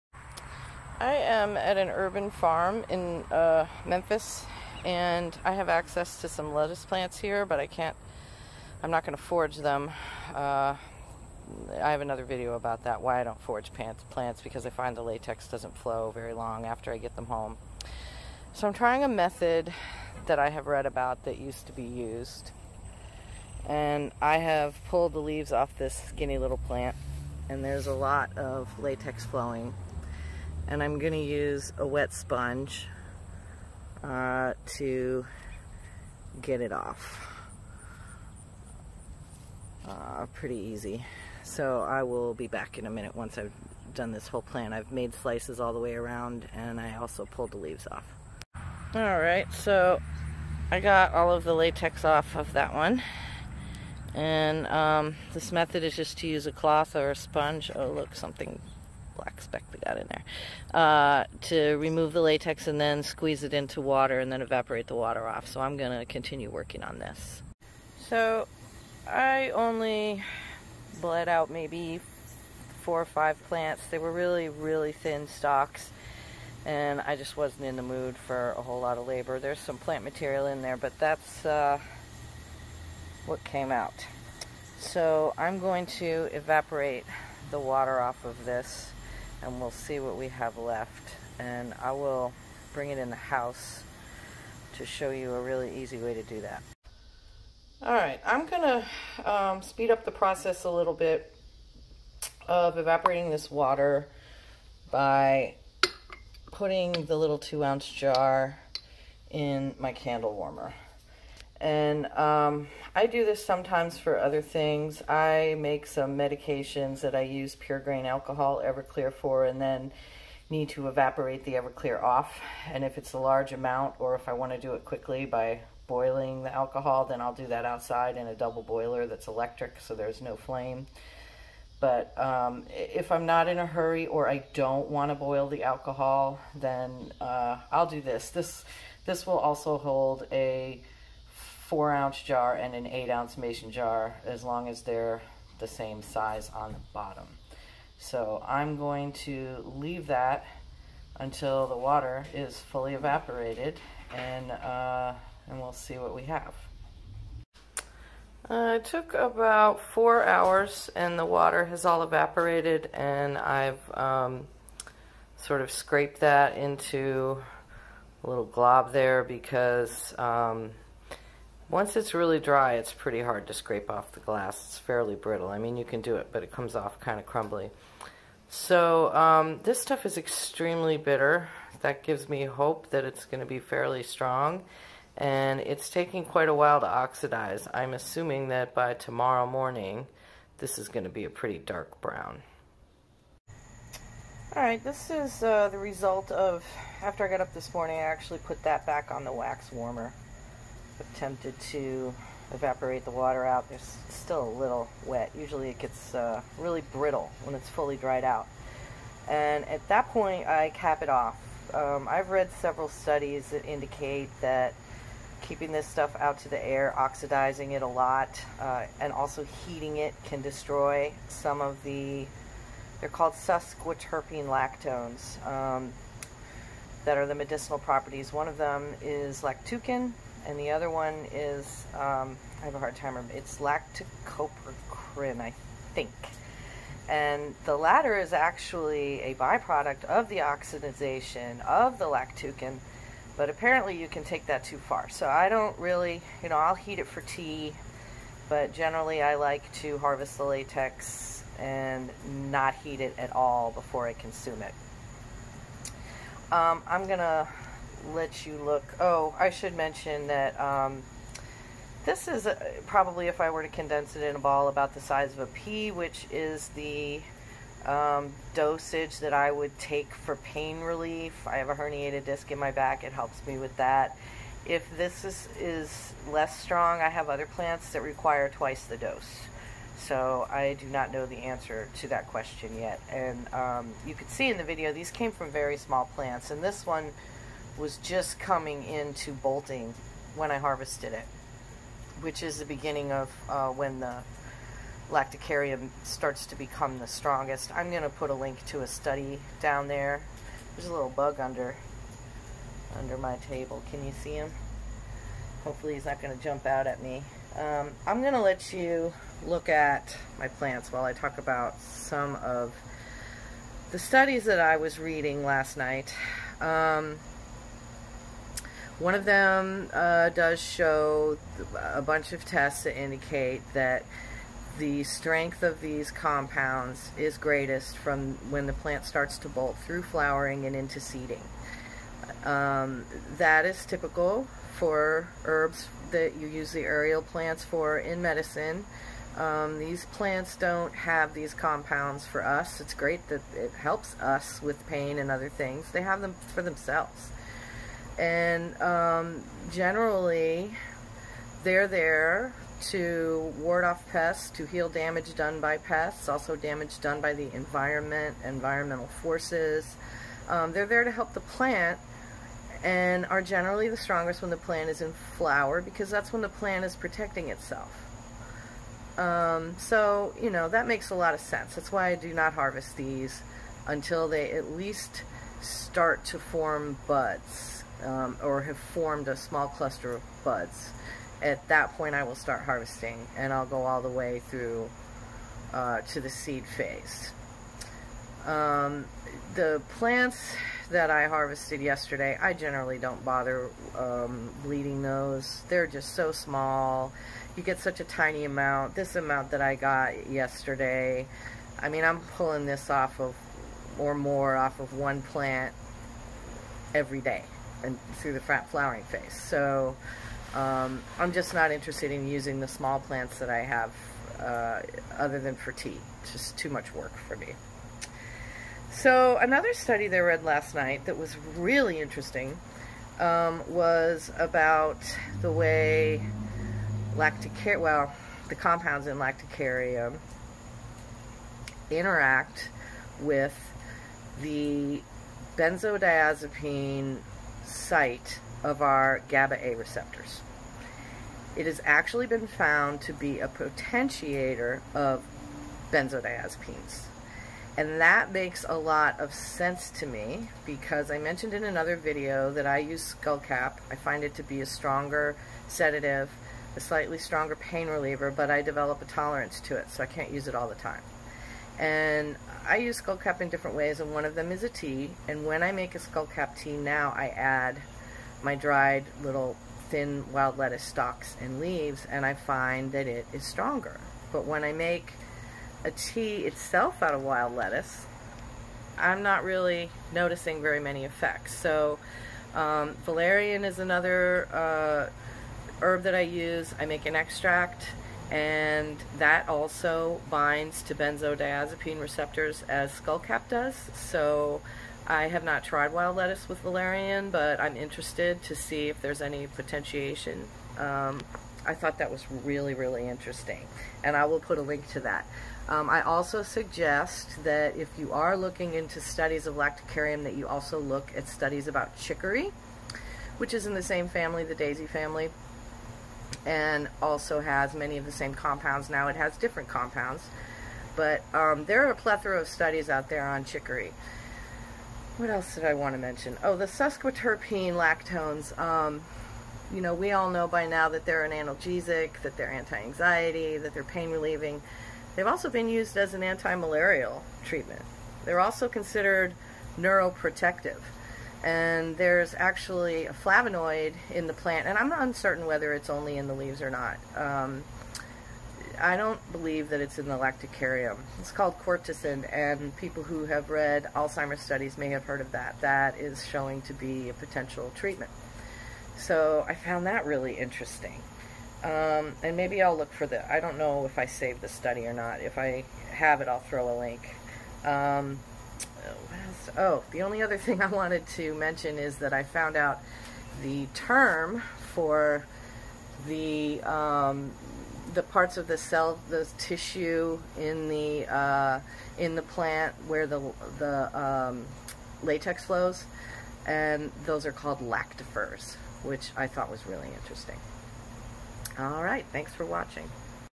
Harvesting the Latex from Lactuca Sativa (Garden Lettuce) with a Sponge
By the time I visited this farm all of the Wild Lettuce plants were beyond the harvesting stage. The plant I'm working on is Lactuca Sativa (cultivated lettuce) that has been allowed to go to seed for several years. I wish I had gotten a picture of it before I tore the leaves off. I don't usually harvest this type of lettuce, but it does create a fair amount lactucarium when left to its own devices. I first learned about its properties from my grandmother who kept a bed in the corner of her yard that she allowed to naturalize, she used it for tea. I found this to be as strong as my weakest wild lettuce plants, which isn't too shabby. In India they use Lactuca Sativa seeds as medicine, it's called Tukhme Kahu. I've linked to a study below that discusses the lactucarium in cultivated lettuce.

* Study: Sesquiterpene Lactone Conjugates in Lactuca

This study shows the strength of sesquiterpene lactones in Lactuca Sativa during the rosette, bolting, flower, and seeded stage. Interestingly, it's a variety of garden lettuce. There is a table of the data in the upper right corner of page 5.

* Study: Potentiating Effects of Lactuca sativa on Pentobarbital-Induced Sleep